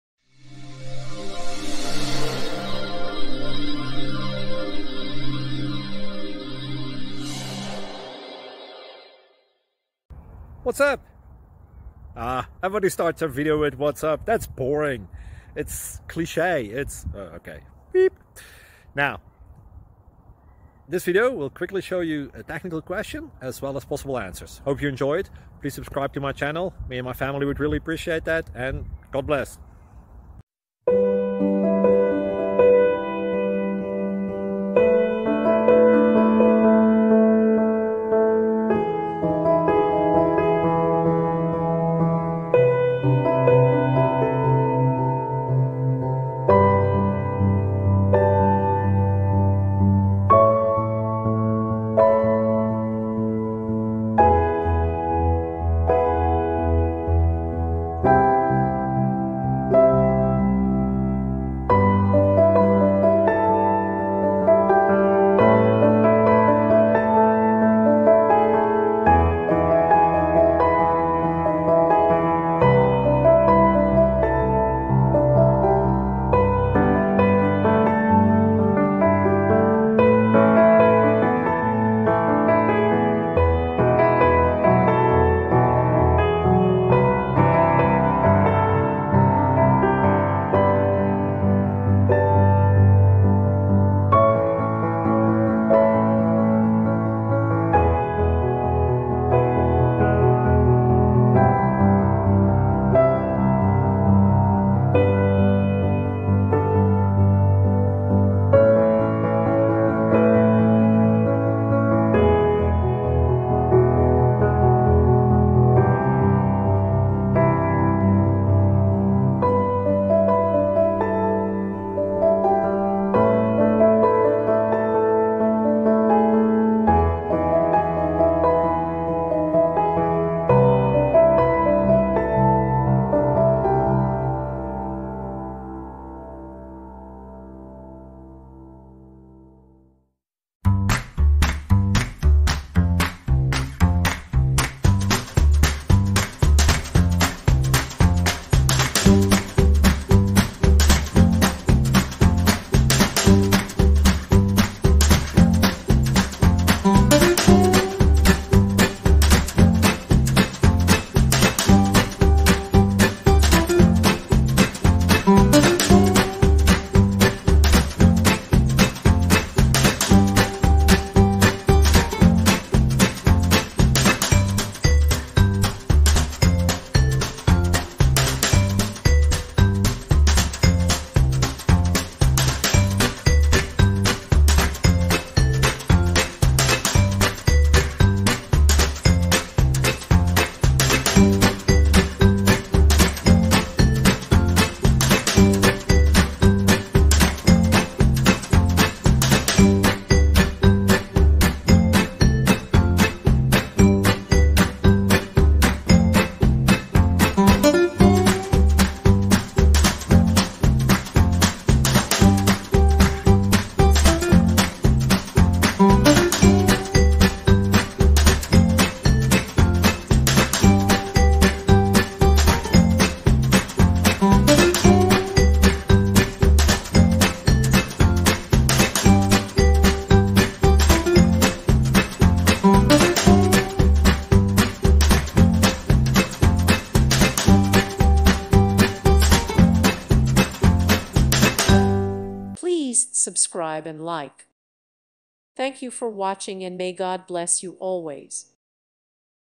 C# pattern to handle "free functions" cleanly, avoiding Helper-style "utility bag" static classes
Software Engineering: C# pattern to handle "free functions" cleanly, avoiding Helper-style "utility bag" static classes

With thanks & praise to God, and with thanks to the many people who have made this project possible! You are responsible for your own actions.